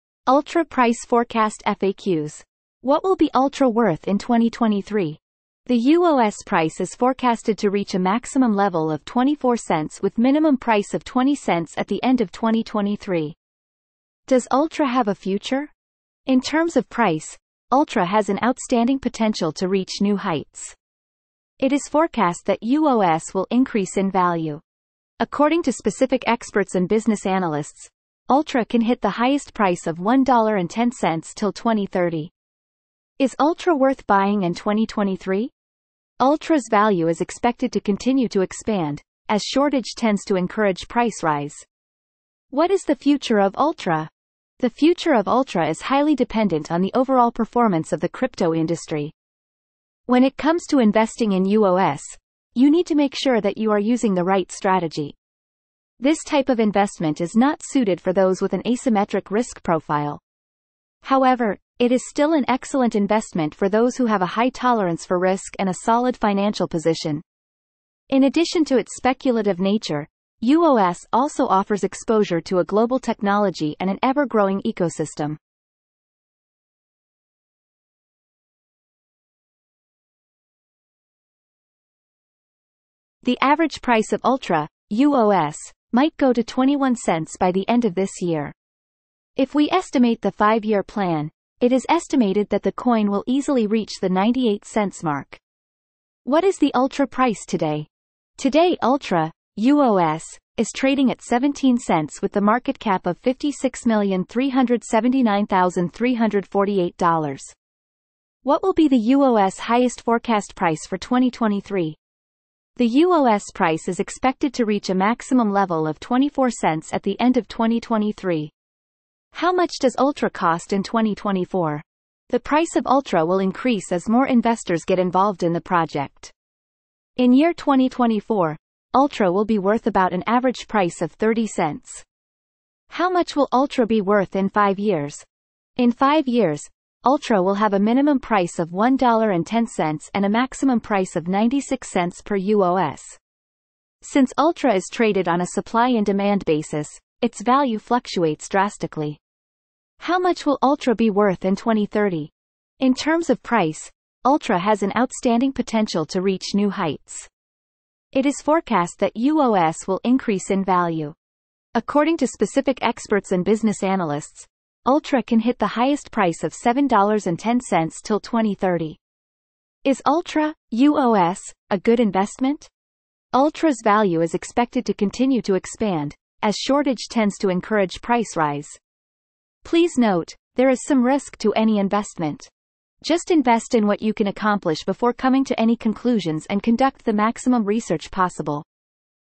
Ultra Price Forecast FAQs
What will be Ultra worth in 2023?
The UOS price is forecasted to reach a maximum level of $0.24 with minimum price of $0.20 at the end of 2023.
Does Ultra have a future?
In terms of price, Ultra has an outstanding potential to reach new heights.
It is forecast that UOS will increase in value.
According to specific experts and business analysts, Ultra can hit the highest price of $1.10 till 2030.
Is Ultra worth buying in 2023?
Ultra's value is expected to continue to expand, as shortage tends to encourage price rise.
What is the future of Ultra?
The future of Ultra is highly dependent on the overall performance of the crypto industry.
When it comes to investing in UOS, you need to make sure that you are using the right strategy.
This type of investment is not suited for those with an asymmetric risk profile.
However, it is still an excellent investment for those who have a high tolerance for risk and a solid financial position.
In addition to its speculative nature, UOS also offers exposure to a global technology and an ever-growing ecosystem.
How high can Ultra go?
The average price of Ultra (UOS) might go to $0.21 by the end of this year.
If we estimate the five-year plan, it is estimated that the coin will easily reach the $0.98 mark.
What is the Ultra price today?
Today Ultra (UOS) is trading at $0.17 with the marketcap of $56,379,348.000.
What will be the UOS highest forecast price for 2023?
The UOS price is expected to reach a maximum level of $0.24 at the end of 2023.
How much does Ultra cost in 2024?
The price of Ultra will increase as more investors get involved in the project.
In year 2024, Ultra will be worth about an average price of $0.30.
How much will Ultra be worth in 5 years?
In five years, Ultra will have a minimum price of $1.10 and a maximum price of $0.96 per UOS.
Since Ultra is traded on a supply-and-demand basis, its value fluctuates drastically.
How much will Ultra be worth in 2030?
According to specific experts and business analysts, Ultra can hit the highest price of $7.10 till 2030.
Is Ultra (UOS) a good investment?
Please note, there is some risk to any investment.
Just invest in what you can accomplish before coming to any conclusions and conduct the maximum research possible.

cryptocurrency, bitcoin, ethereum, crypto, mining, trading, sec, bullrun, bearish, wallet, prediction, forecast